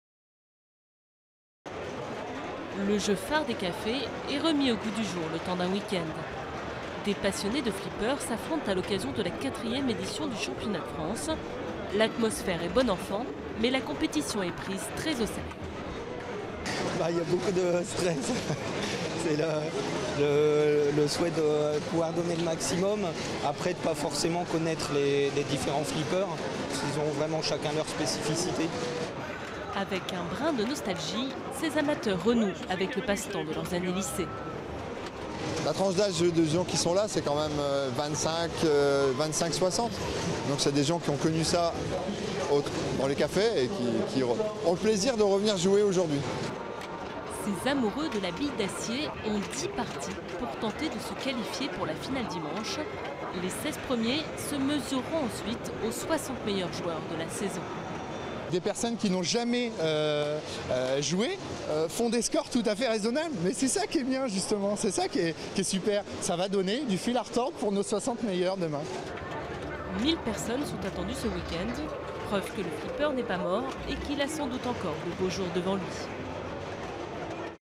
Des passionnés s'affrontent au championnat de France de flipper
Disparu des cafés de France, le flipper connaît ce weekend son heure de gloire avec les 4e championnats de France organisés dans le Pas de Calais à Carvin. Un tournoi auquel vont tenter de se qualifier des quidam. Seize d'entre eux rejoindront les 60 meilleurs Français dans la phase finale prévue dimanche. SUJET. Durée: 01:18